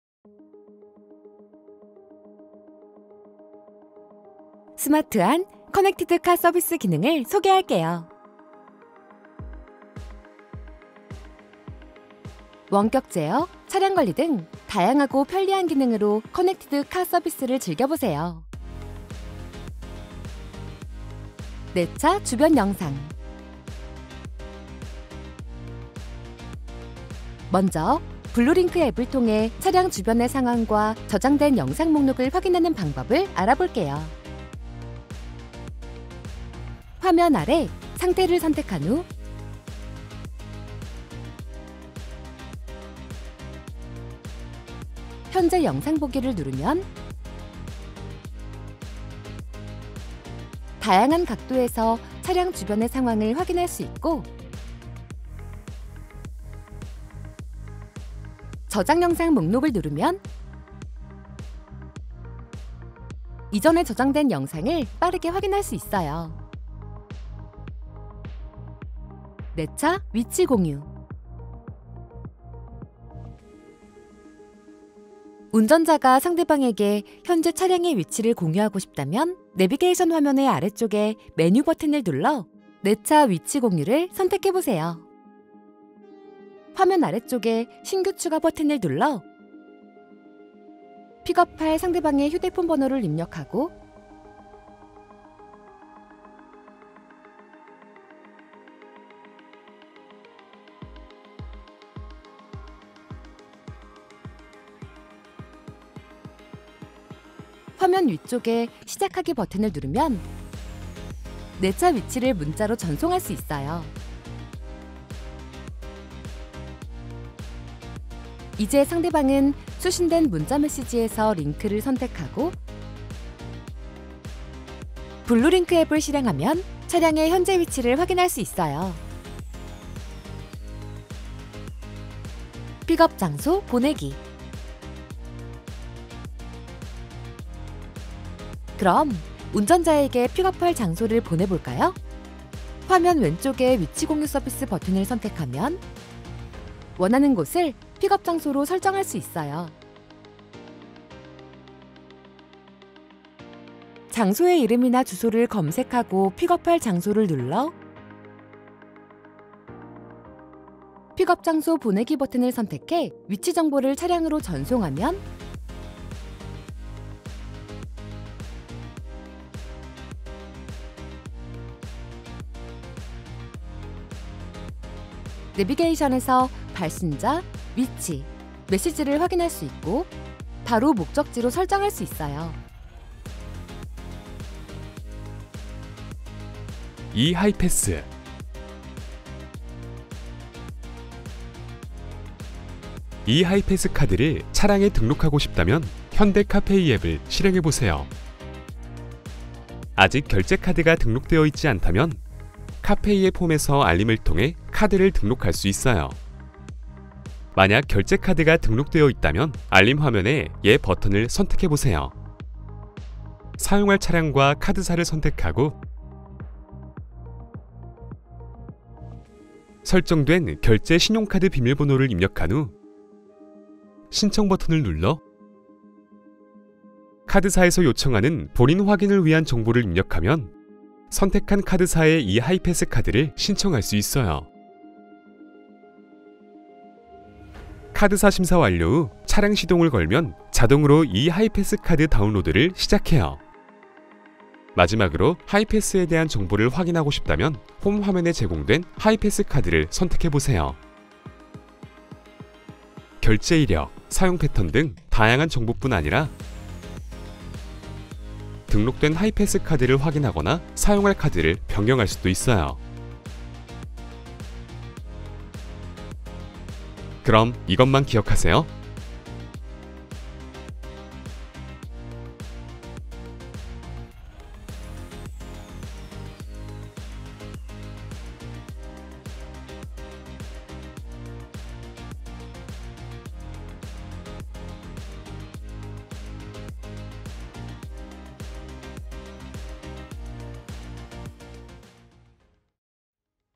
현대자동차 커넥티드 카 서비스 기능 사용법 | 디 올 뉴 그랜저, 디 올 뉴 코나, 쏘나타 디 엣지
현대자동차 ccNC (connected car Navigation Cockpit) 커넥티드 카 서비스 기능 사용 방법을 알려드릴게요.
디 올 뉴 그랜저 (The all-new GRANDEUR), 디 올 뉴 코나 (The all-new KONA), 쏘나타 디 엣지 (SONATA The Edge) 등 에서 만나보실 수 있습니다.

▶ 더 자세히 알아보기.

00:00 인트로
00:10 서비스 소개
00:20 내 차 주변 영상
01:08 내 차 위치 공유
02:12 픽업 장소 보내기
04:25 Summary